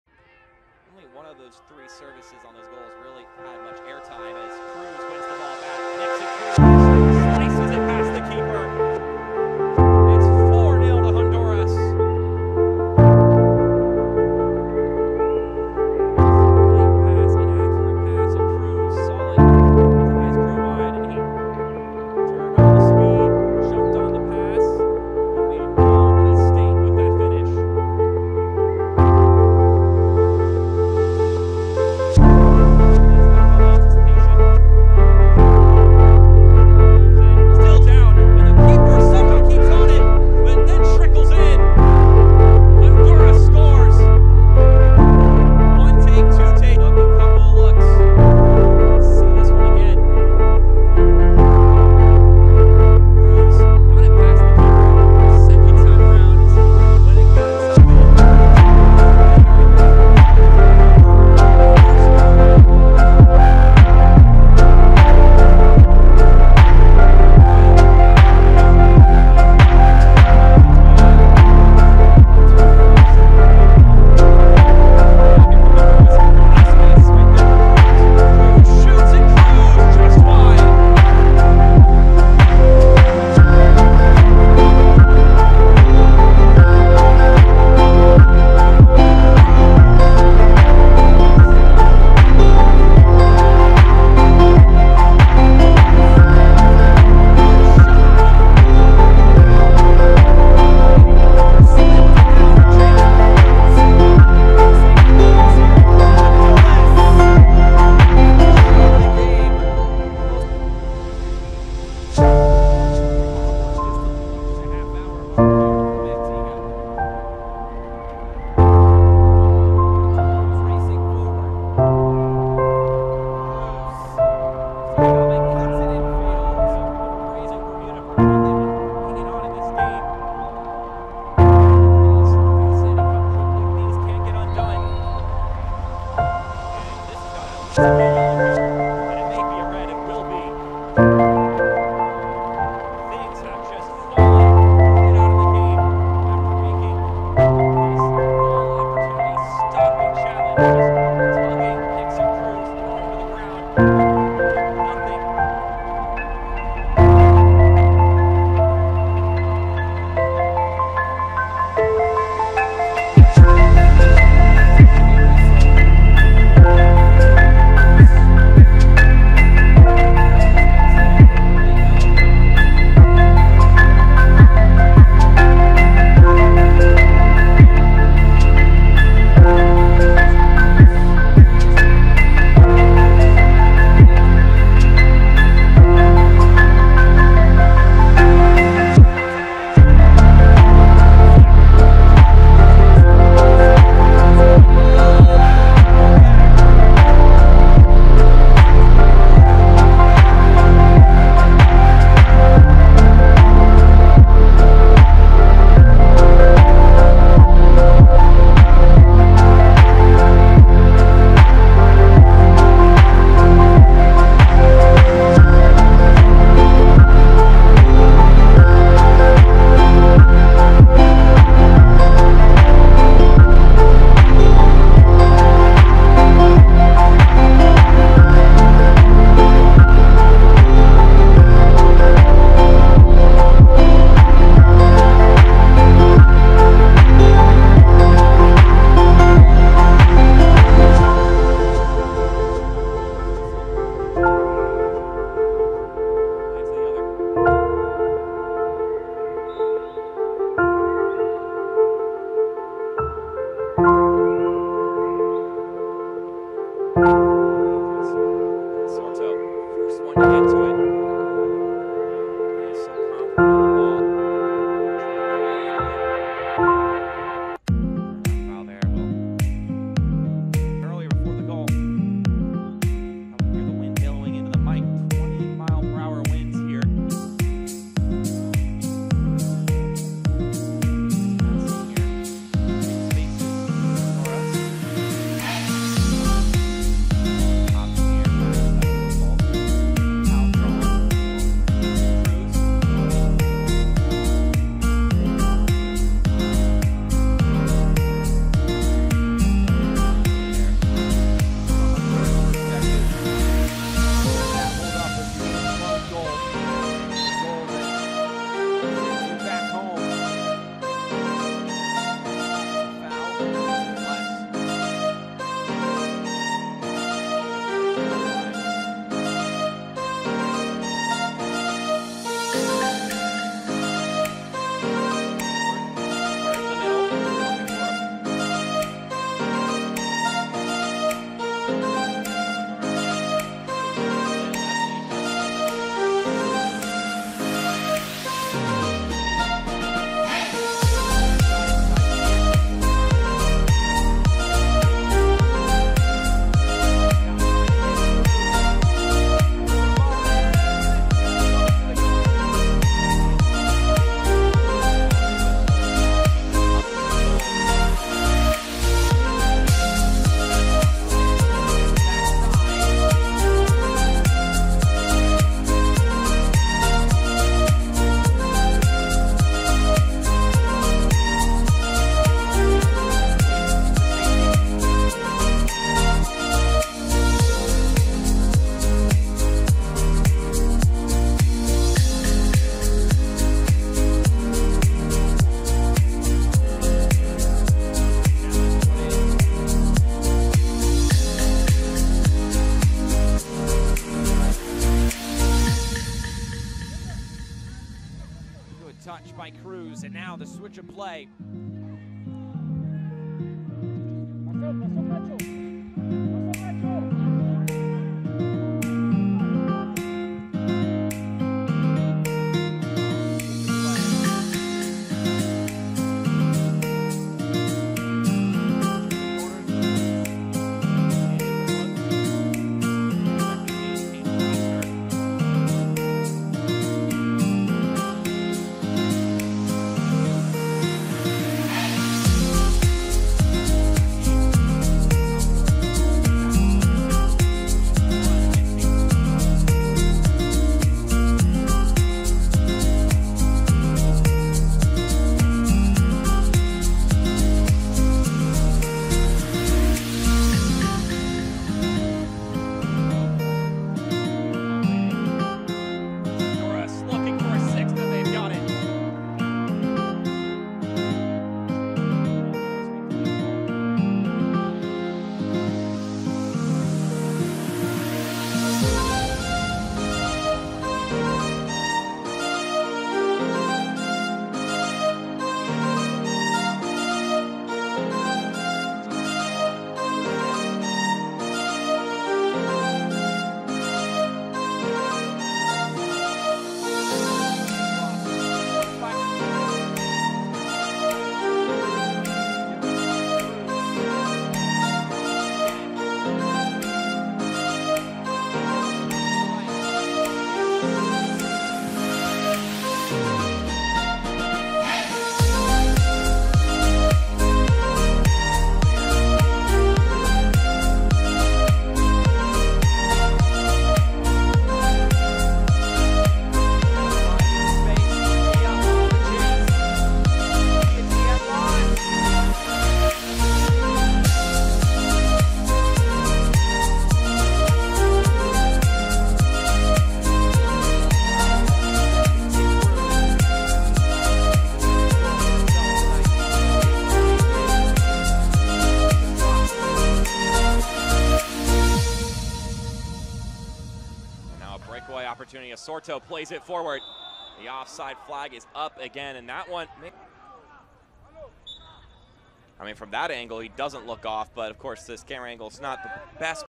Nixon Cruz. Honduras Winger 16 years old in CONCACAF Under-17
The 2023 CONCACAF U-17 Championship was the 7th edition of the CONCACAF Under-17 Championship (20th edition if all eras included), the men's under-17 international football tournament organized by CONCACAF.[It was hosted by Guatemala between 11 February and 26 February 2023. The top four teams qualified for the 2023 FIFA U-17 World Cup in Indonesia as CONCACAF representatives.
The 41 CONCACAF teams were ranked based on the CONCACAF Men's Under-17 Ranking as of December 2019. A total of 38 teams entered the tournament. The sixteen highest-ranked entrants were exempt from qualifying and advanced directly to the group stage of the final tournament, while the lowest-ranked twenty-two entrants had to participate in qualifying, where the four group winners advanced to the round of 16 of the knockout stage of the final tournament.